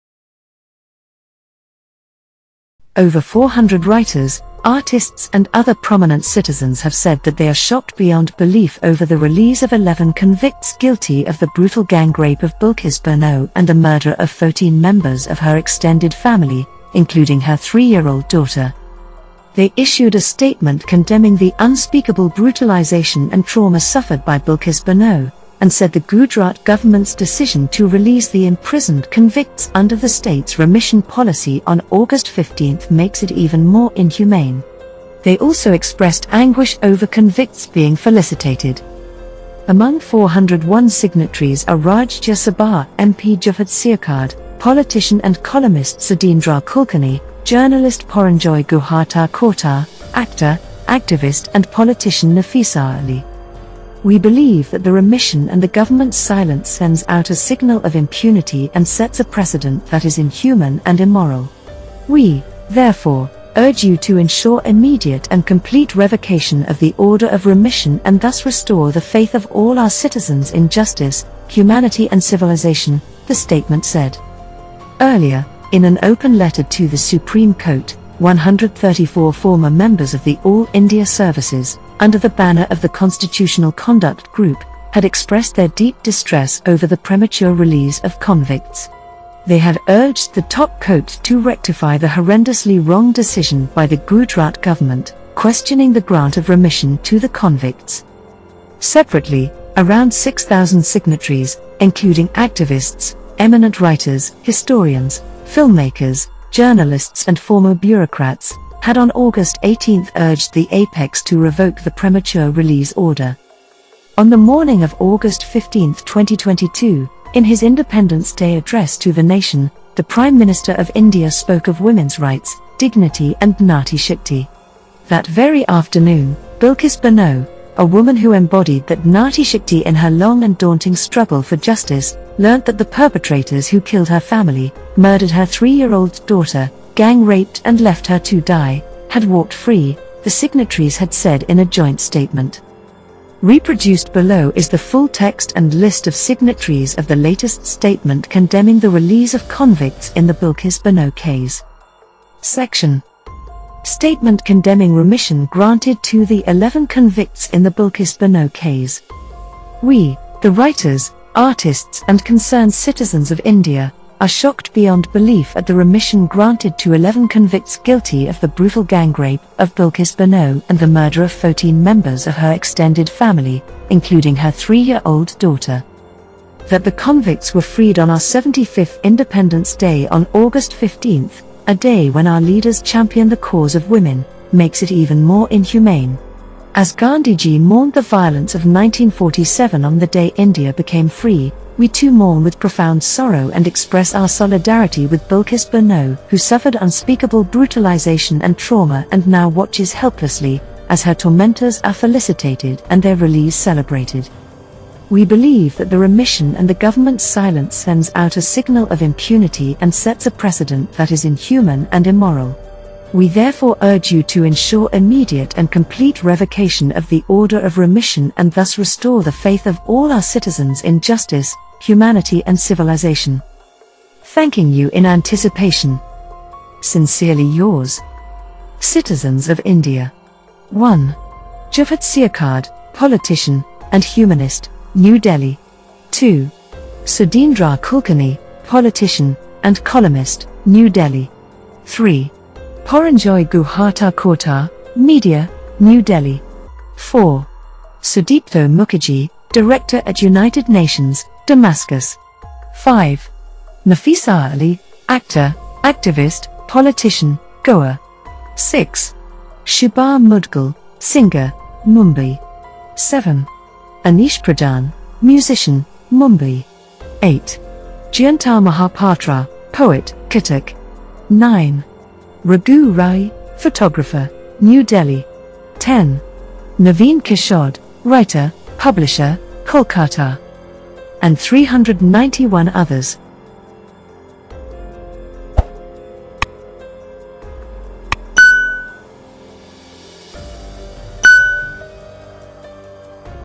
Signal of Impunity: Over 400 Writers, Artists Decry Release of Convicts in Bilkis Bano Case
In a statement, Jawhar Sircar, Sudheendra Kulkarni, Paranjoy Guha Thakurta and others called on the government to revoke the early release of the convicts, saying that it "sets a precedent that is inhuman and immoral".

Over 400 writers, artists and other prominent citizens have said that they are “shocked beyond belief” over the release of 11 convicts guilty of the brutal gangrape of Bilkis Bano and the murder of 14 members of her extended family, including her three-year-old daughter.